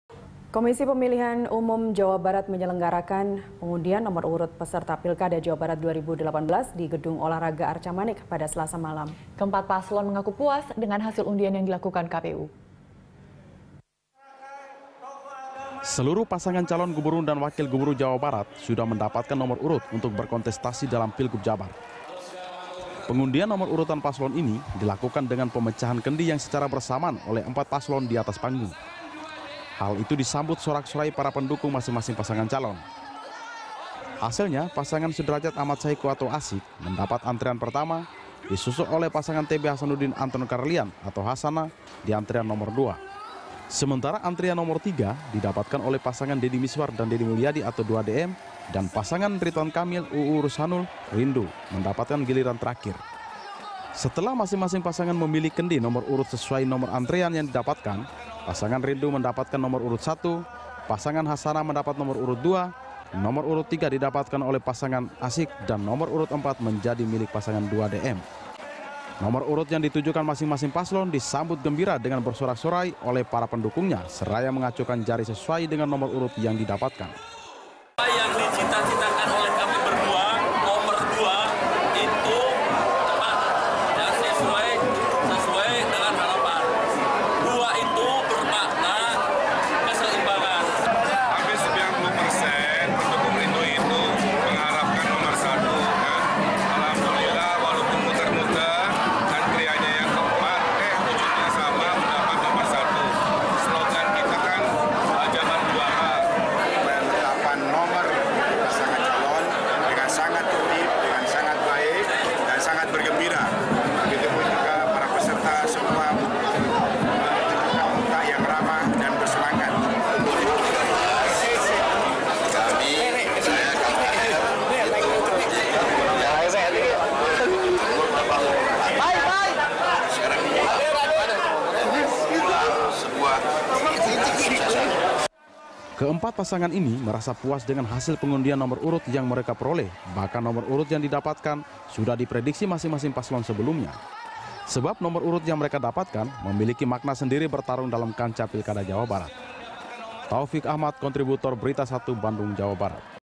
4 Paslon Gubernur Jabar Puas Hasil Pengundian Nomor Urut
Komisi Pemilihan Umum (KPU) Jawa Barat menyelenggarakan pengundian nomor urut peserta Pilkada Jawa Barat 2018 di Gedung Olahraga Arcamanik Selasa (14/02/2018) malam. Ke-empat pasangan calon mengaku puas dengan hasil undian yang dilakukan KPU.

Facebook.com/BeritaSatuTV
Youtube.com/BeritaSatu